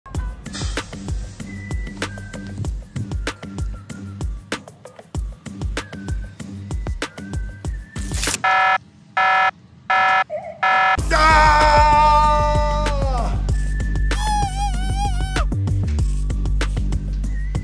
Footballers ALL know this pain...
When you forget to shave your leg before taping it up... A pain all football players will know when they arrive at the dreaded moment of pulling the tape off and also pulling out half of the hairs on your leg. The pain is real.